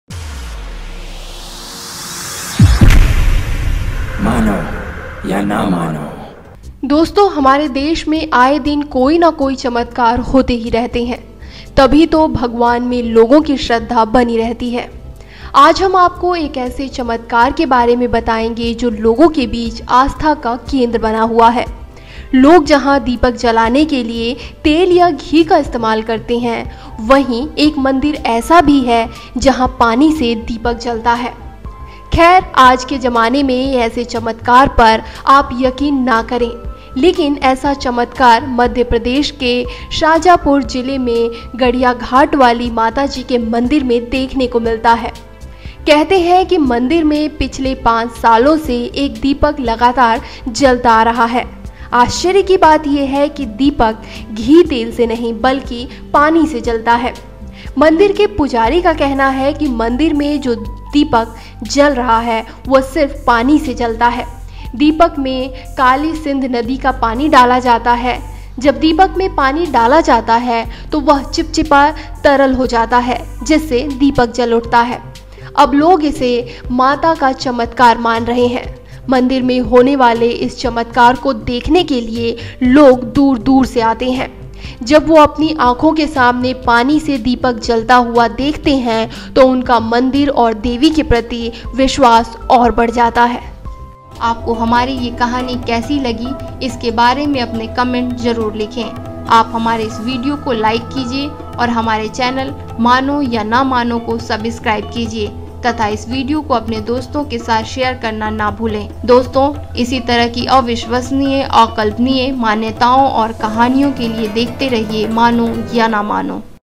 अनोखा चमत्कार, 5 साल से इस मंदिर में पानी से ही जल रहा है दीपक | Pani Ka Diya | MIRACLE AND MYSTREY
जहां मंदिर में दीपक जलाने के लिए तेल या घी का इस्तेमाल करते है, वहीं एक मंदिर ऐसा भी है जहां पानी से दीपक जलता है।ख़ैर आज के ज़माने में ऐसे चमत्कार पर आप यकीन न करें लेकिन ऐसा ही चमत्कार मध्य प्रदेश के शाजापुर ज़िले में कालीसिंध नदी के किनारे स्थित गडिय़ाघाट वाली माताजी के मंदिर में देखने को मिलता है। कहते हैं कि मंदिर में पिछले 5 सालों से एक दीपक लगातार जलता आ रहा है। आश्चर्य की बात ये है कि दीपक घी-तेल से नहीं बल्कि पानी से जलता है।